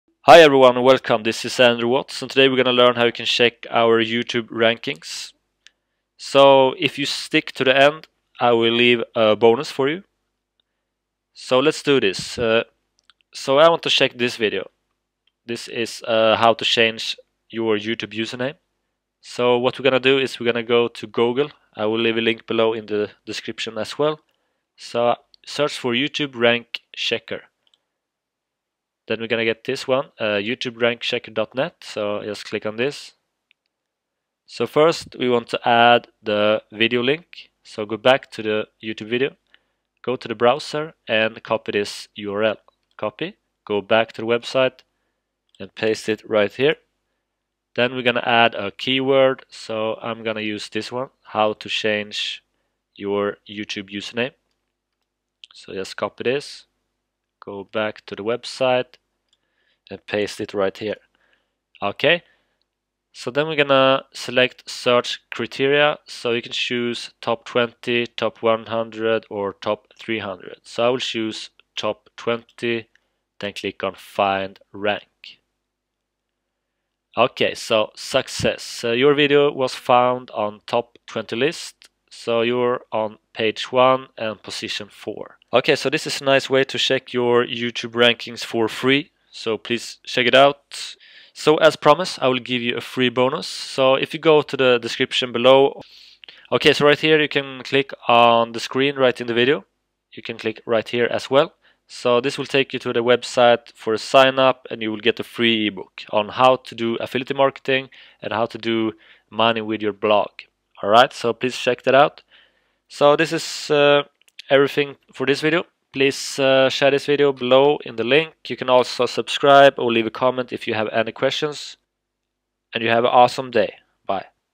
How to Check Youtube video Ranking for Free 2016
Learn how to check Youtube video ranking on your videos for free with this easy step by step tutorial 2016.

With this youtube rank checker website you don't get the wrong ranking position.

If you search yourself on your own computer you can get he wrong ranking position because you may have watch your own video before.

FOLLOW ME

Snapchat. Andrew Waatz